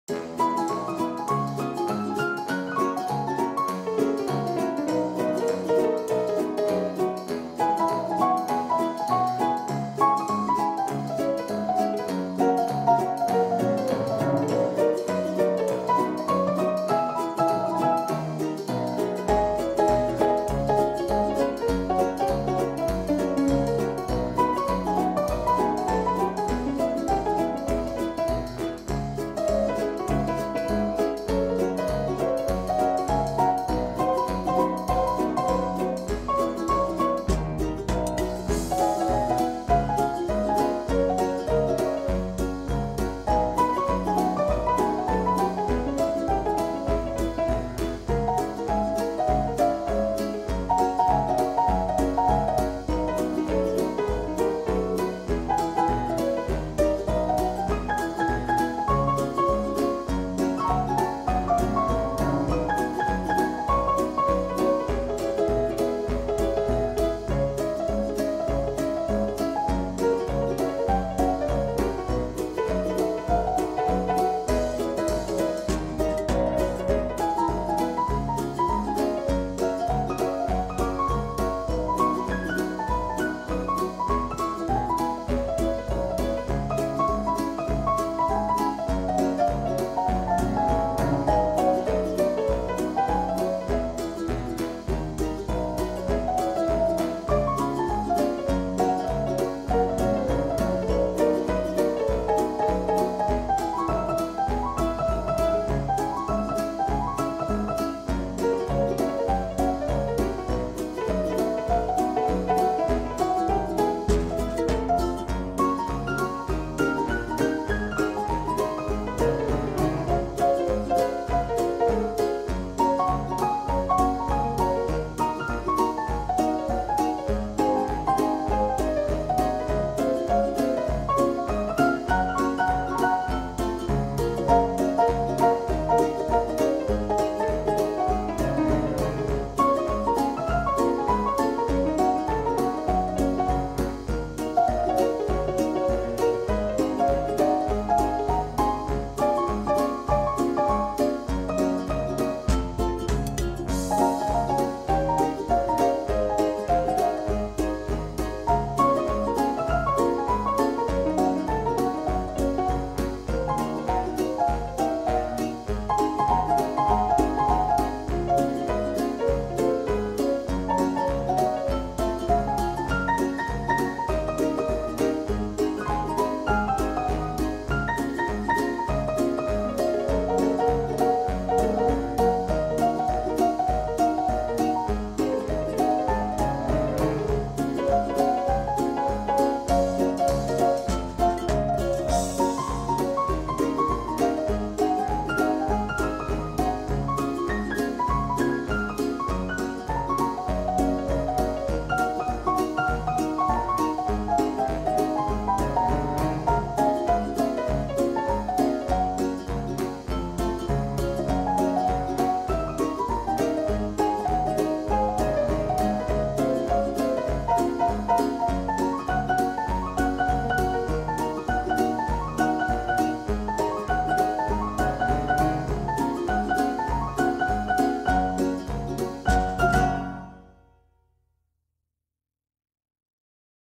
Zumbrota Covered Bridge Old Movie
I found an old time movie effect in my video editing software.  I thought it would fit with the music and the subject of this video.